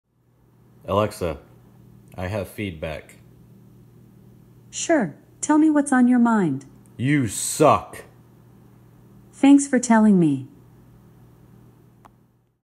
Alexa I Have Feedback
In this video, I will be giving Alexa my honest feedback.